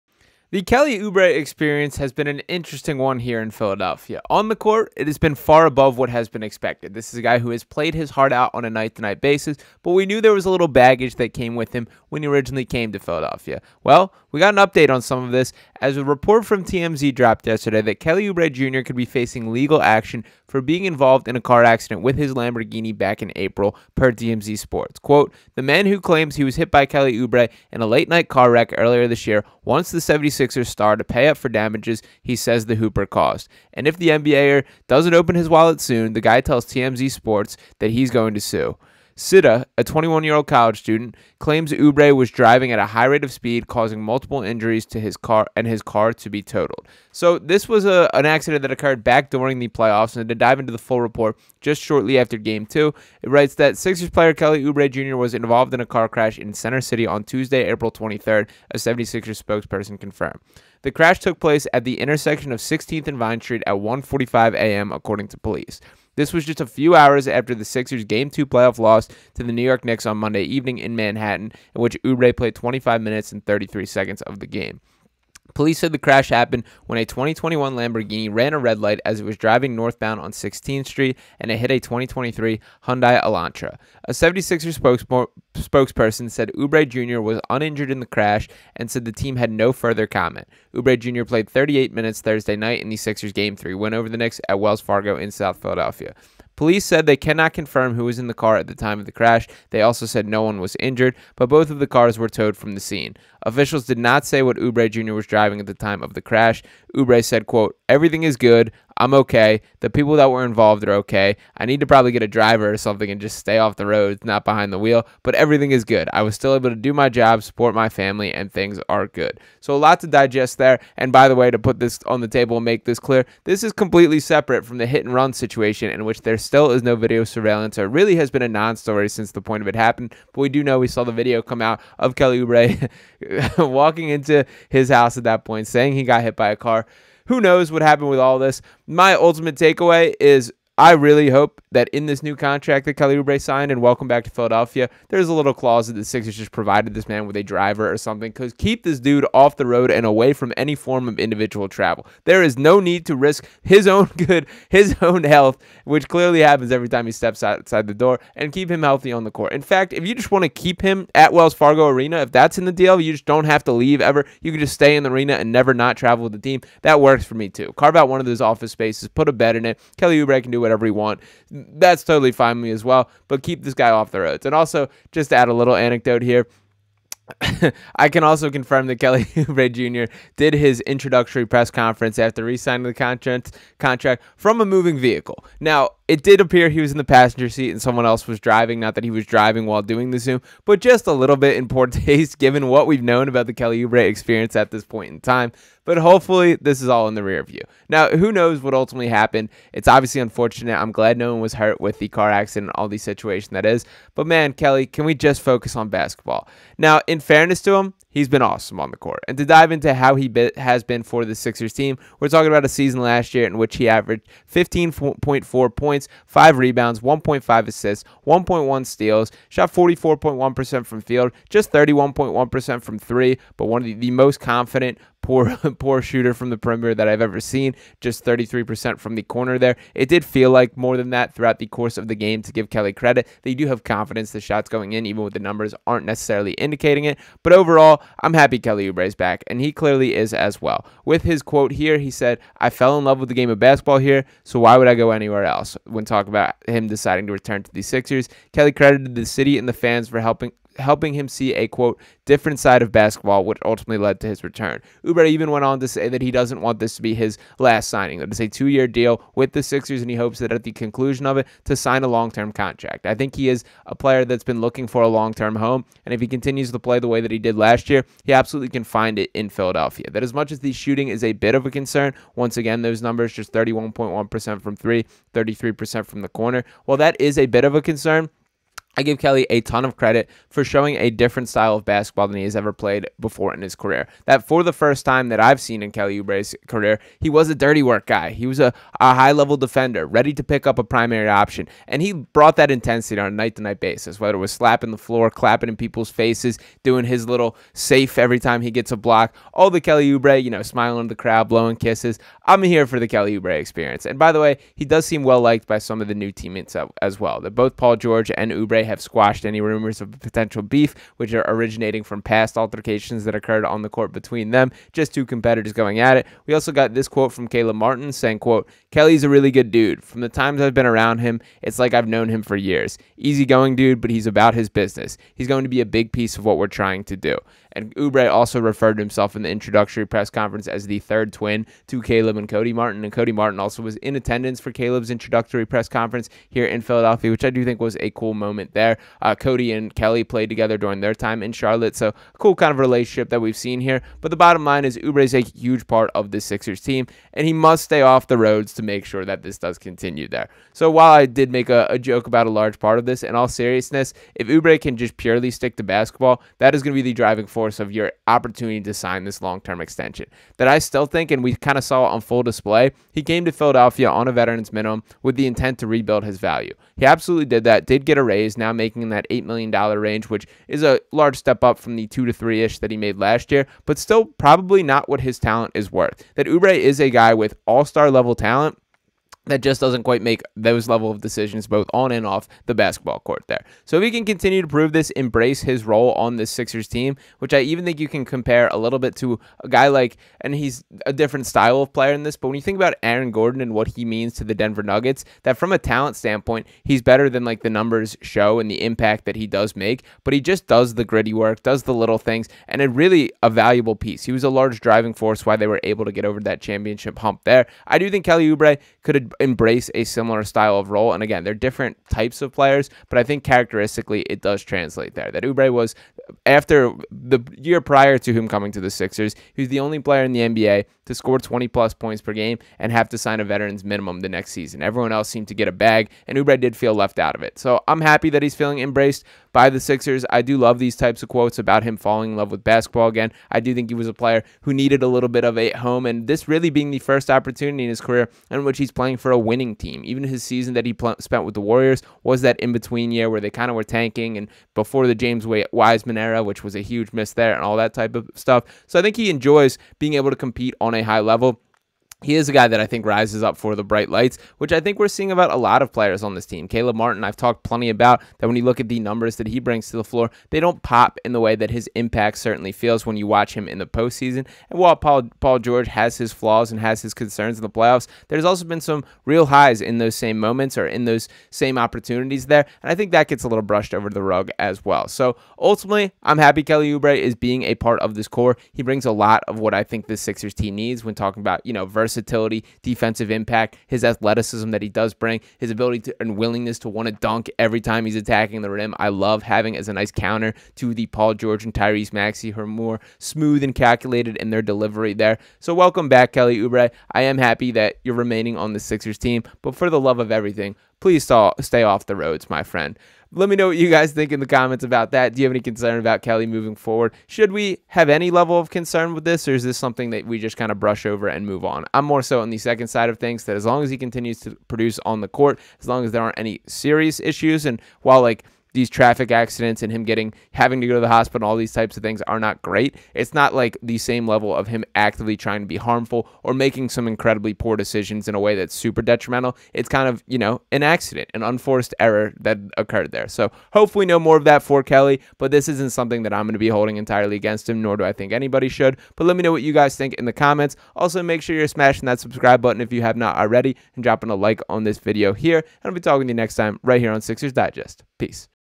WILD Kelly Oubre Jr. Update...
There is never a dull moment in the Kelly Oubre Jr experience. Shortly after re-signing with the Sixers, a report surfaced saying that Oubre may be sued for a car accident dispute that occurred last year following a Sixers playoff loss to the Knicks. Sean Barnard breaks down what went down and what to make of all of this heading into the season.